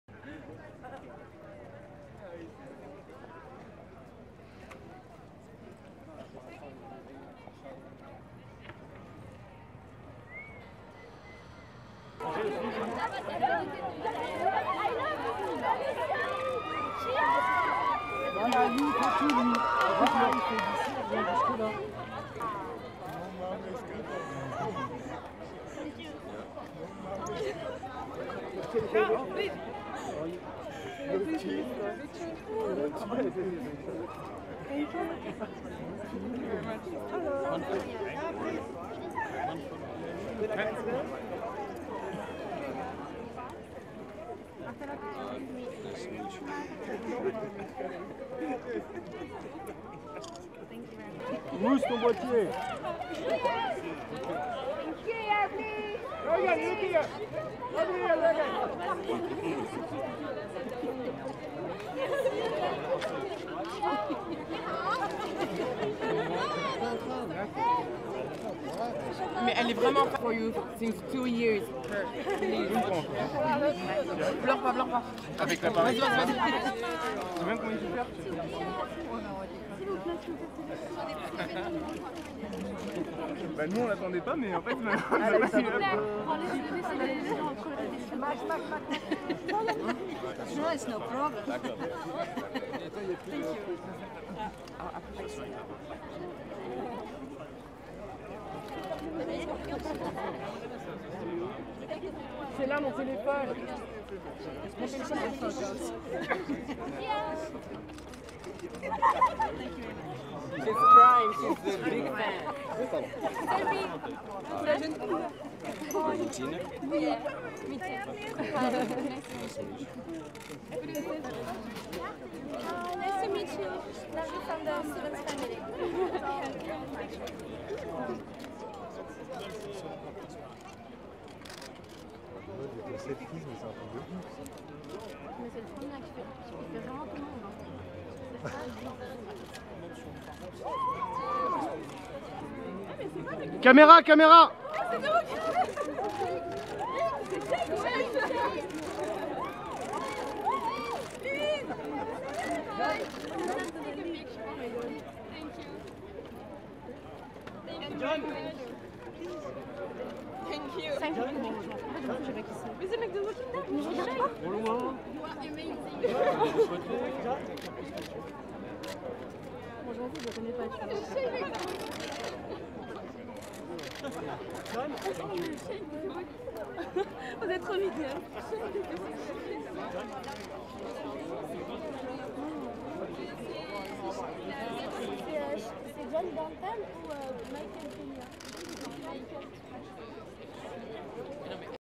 Shia LaBeouf greets fans at Fury Photocall in Paris
American actor Shia LaBeouf greets fans at Fury Photocall in Paris
Paris, France on Saturday October 18, 2014